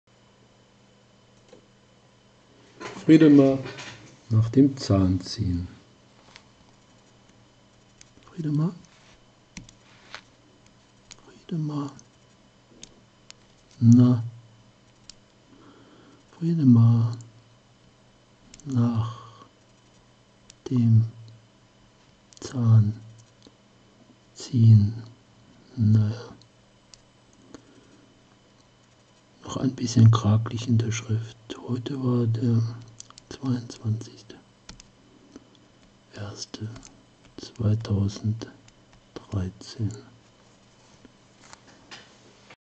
NachZahnziehen
Fridemar, heute nach dem Ziehen seines
ersten Backenzahns